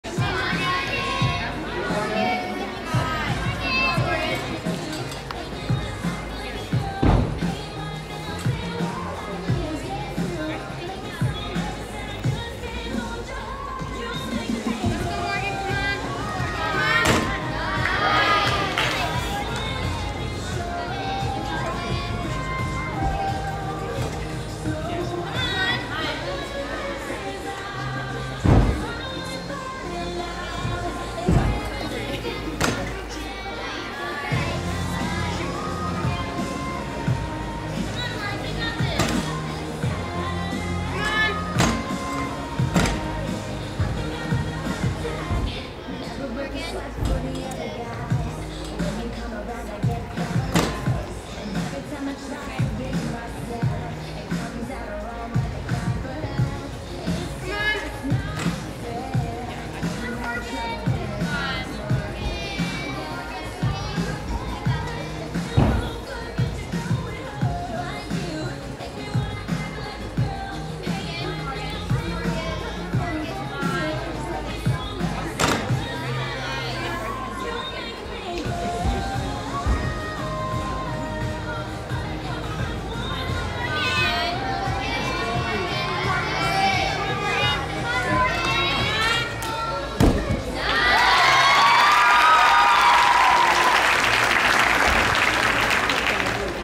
Morgan Hurd - Balance Beam - 2018 World Team Selection Camp
Oct. 11, 2018 - EVO Athletics - Sarasota, Fla.